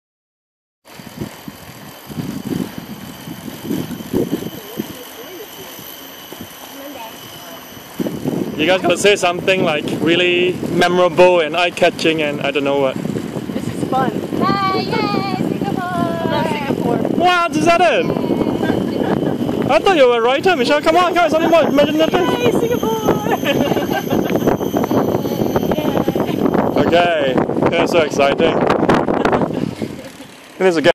Riding Bikes on Pulau Ubin, Singapore
Just enjoying a little tandem bike rides around the trails of Pulau Ubin, an island off of Singapore.